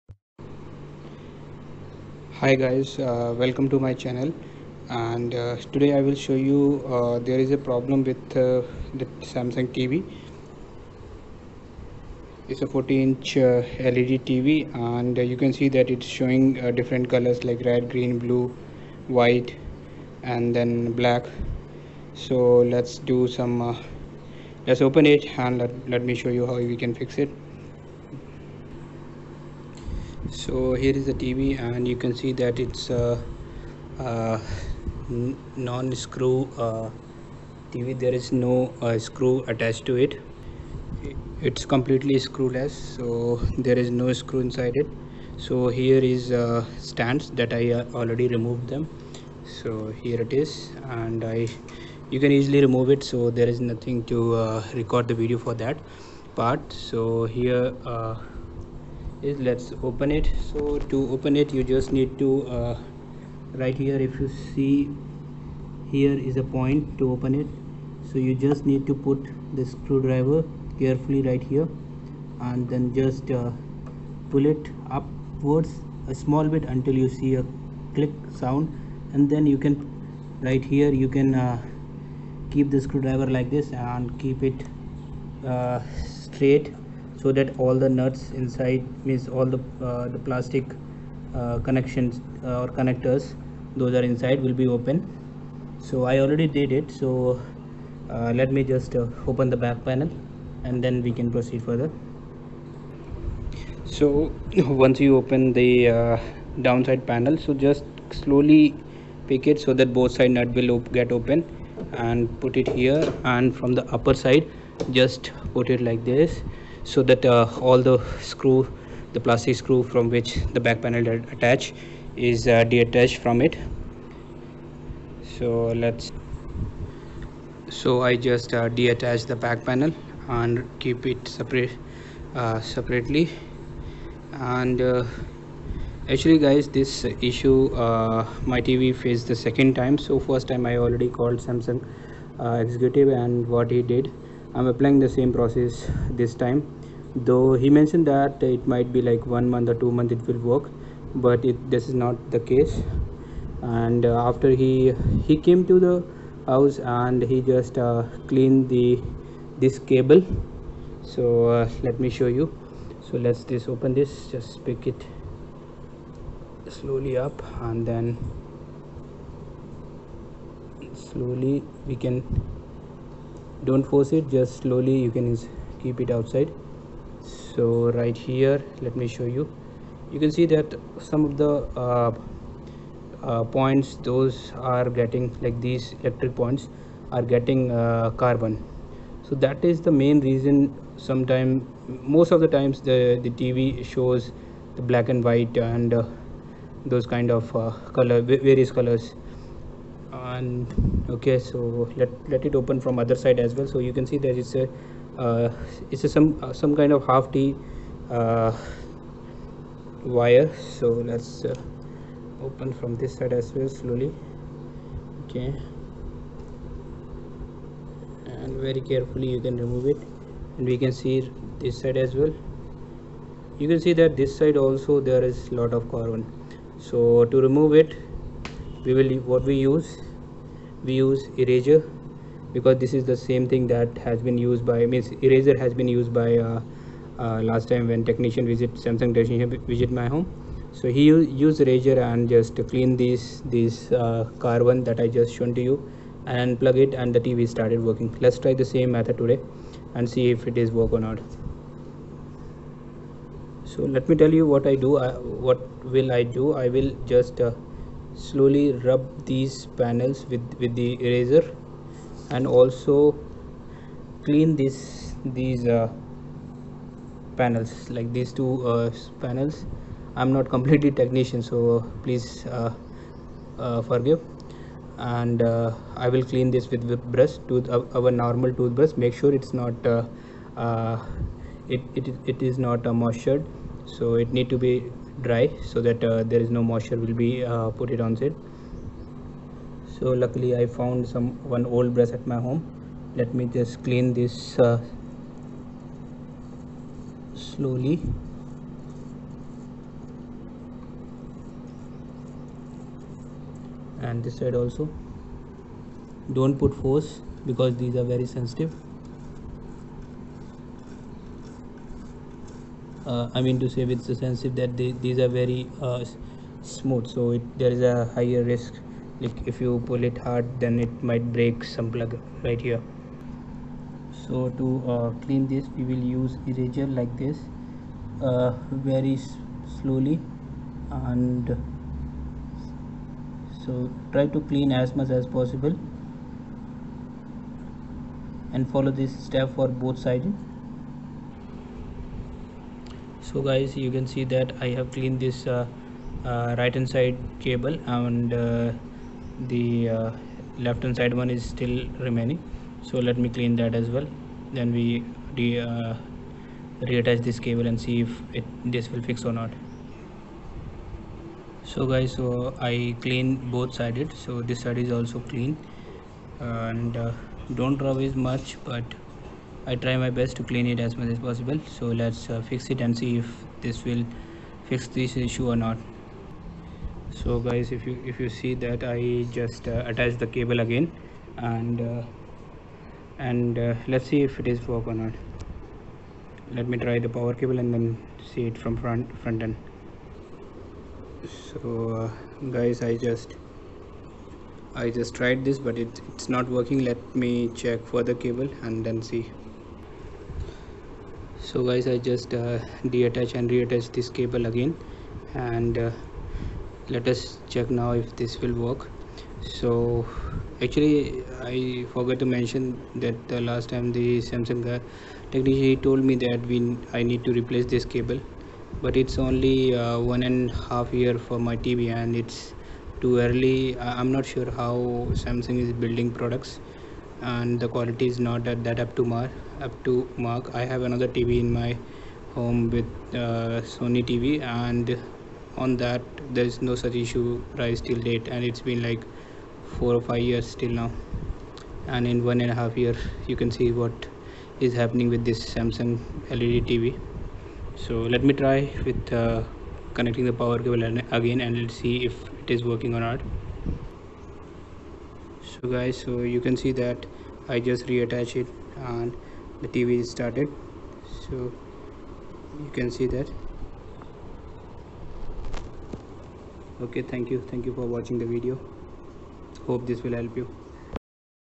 Samsung LED TV Solid Colors Burning Mode Issue - Blue, Red, Green, White & Black- SOLVED
Samsung LED TV solid colors problem - blue, red, green, white, black solid color - Led tv burning mode issue fix.

led tv burning mode problem
burn mode in led tv